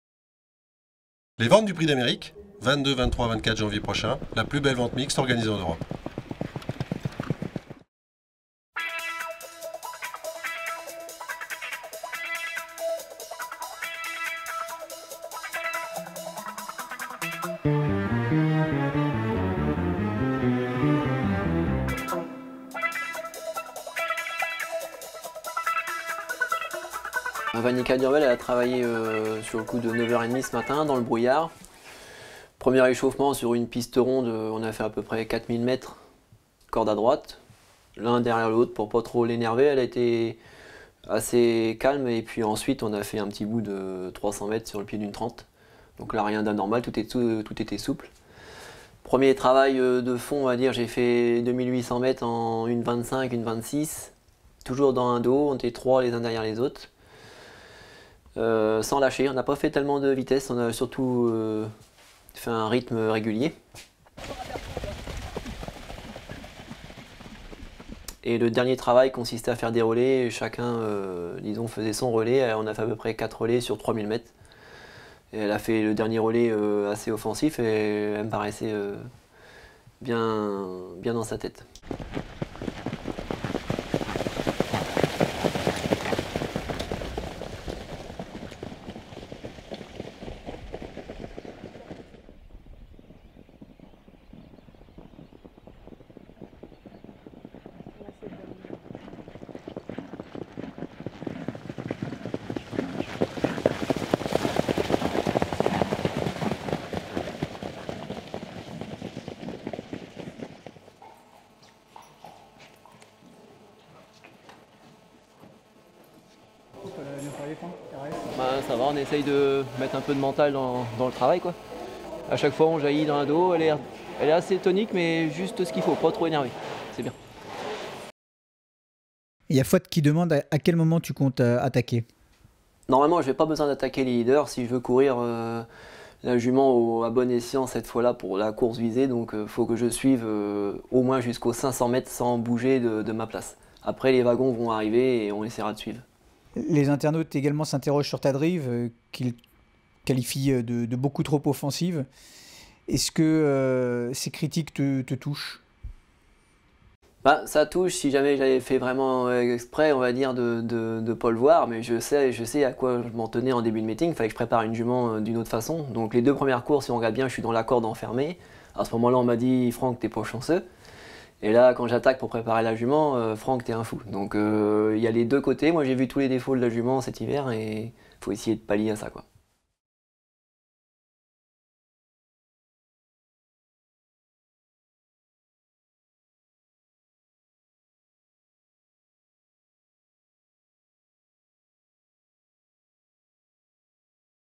A 5 jours du Prix d'Amérique 2014, VANIKA DU RUEL au travail
A 5 jours du Prix d'Amérique 2014, nous nous sommes rendus en Normandie pour assister à l'un des derniers exercices de VANIKA DU RUEL avant l'évènement.
Quelques images et l'interview de Franck ANNE avant un grand format dans les jours à venir.